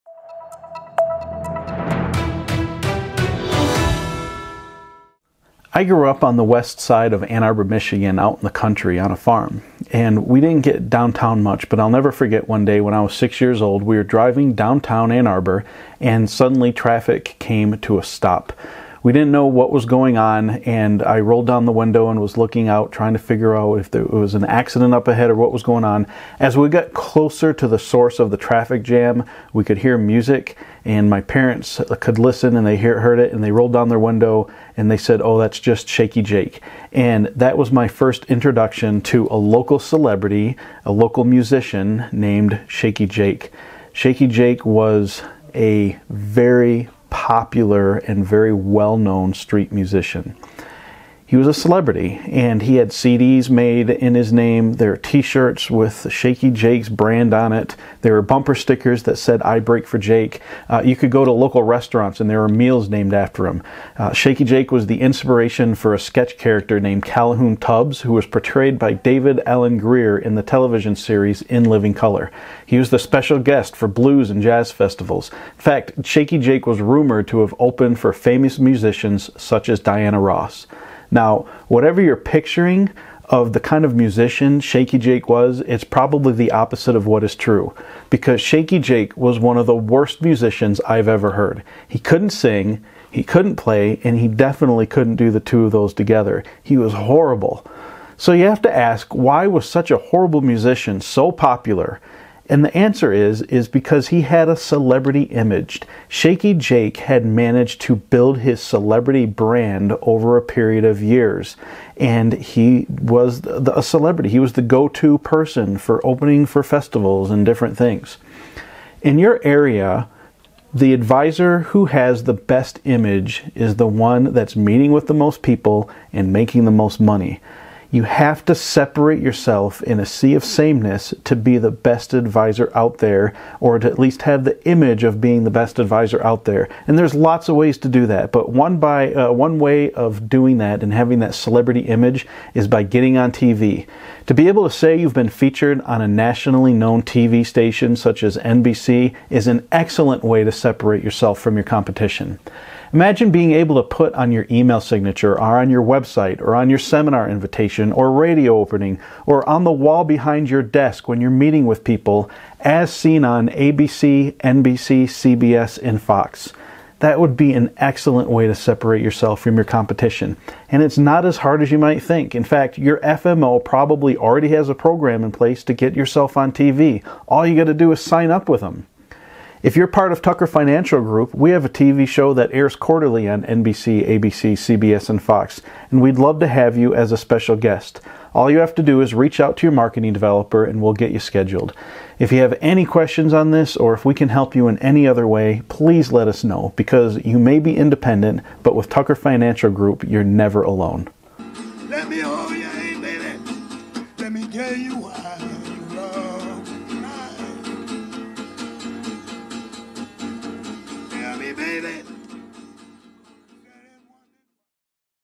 Justin's Marketing Corner: Building Your Celebrity Image
Shakey Jake was not the best musician, but he had the best image. Have you separated yourself from your competition? In today's video, Justin talks about how you can bring positive attention to your financial advisory practice while separating yourself from the advisors in your area.